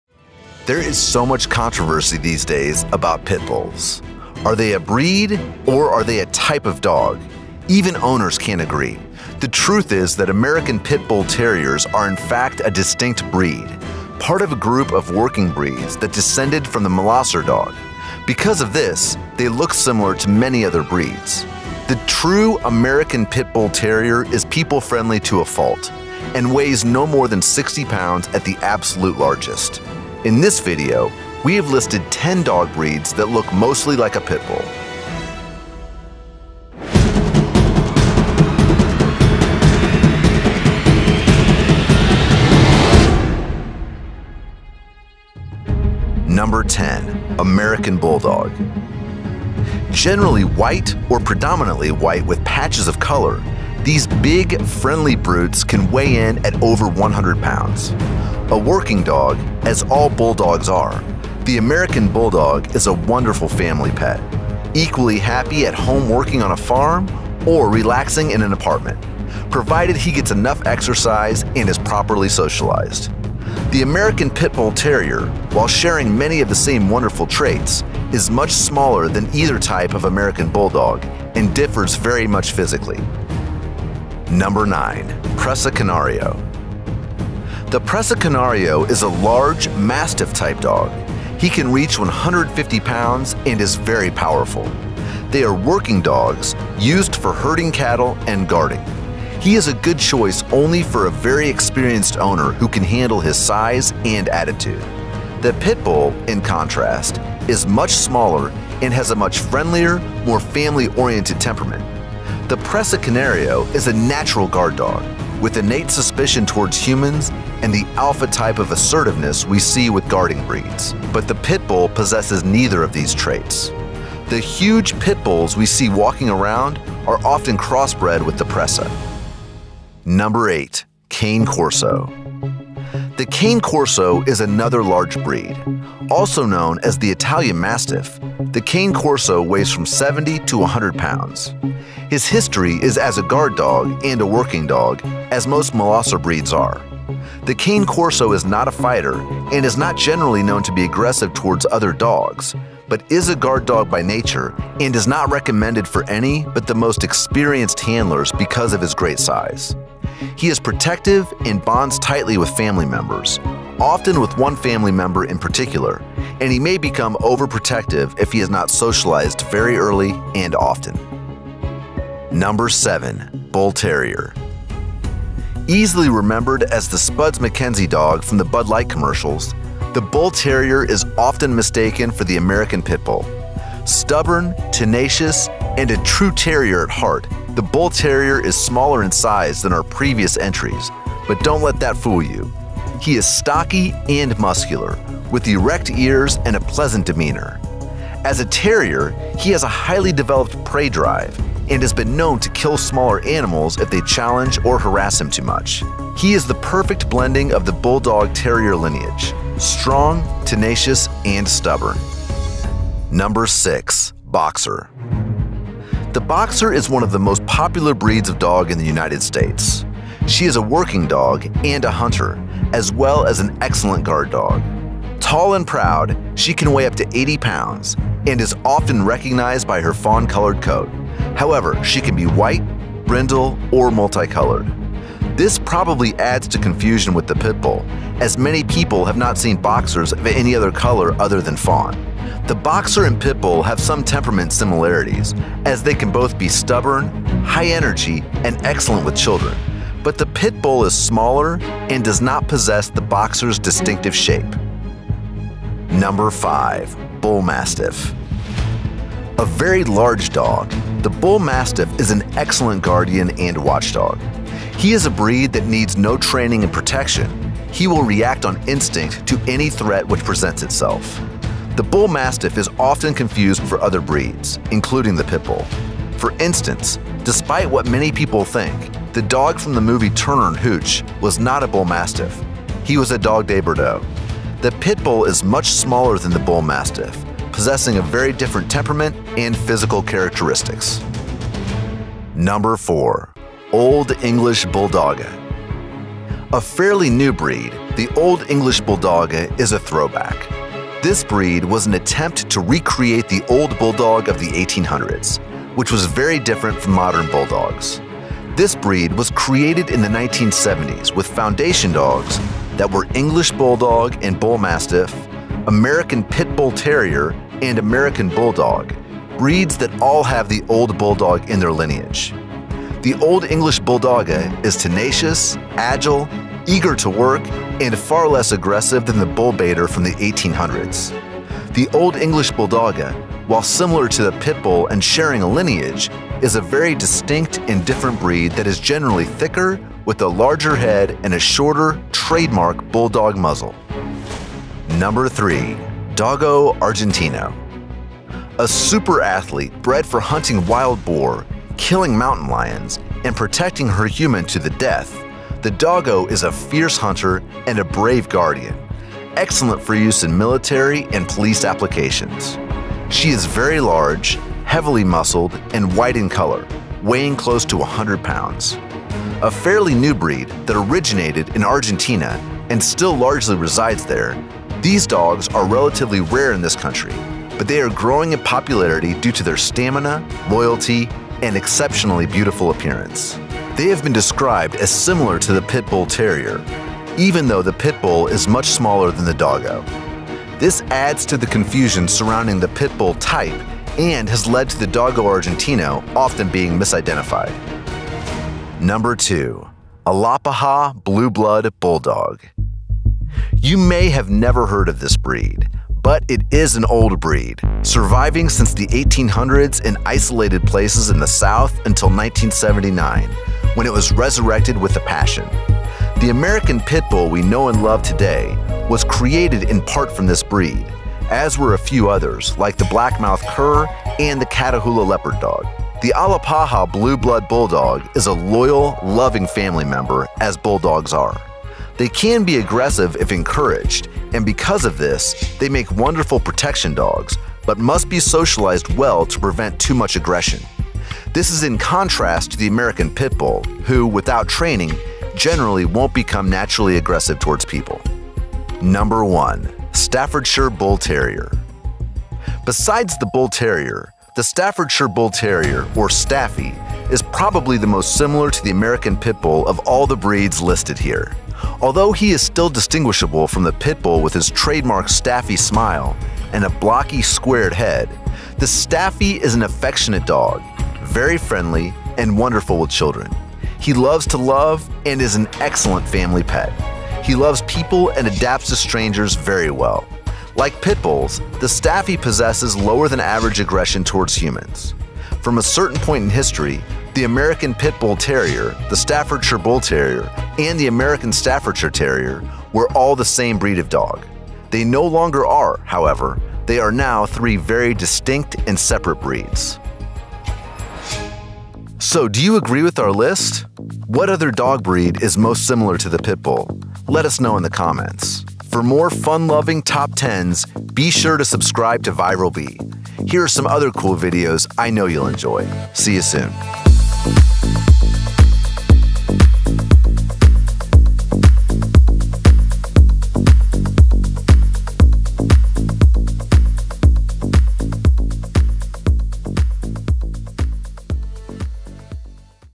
These 10 Dog Breeds Look Like Pitbulls
There is so much controversy these days about pit bulls. Are they a breed or are they a “type” of dog? Even owners can’t agree.
The truth is that American pit bull terriers are in fact a distinct breed,
part of a group of working breeds that descended from the Molosser dog.
Because of this, they look similar to many other breeds.
The true American pit bull terrier is people-friendly to a fault and weighs no more than 60 pounds at the absolute largest.
In this video, we have listed 10 dog breeds that look mostly like a pitbull.